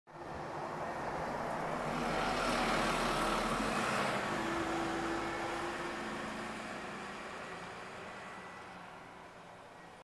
Mercedes Citaro C2 BSU Solothurn (Soleure)
Mercedes Citaro C2 n° 59 du réseau urbain BSU de Solothurn (Soleure) dans le canton de Soleure en Suisse Alémanique sur la ligne 5 le 07/07/2020.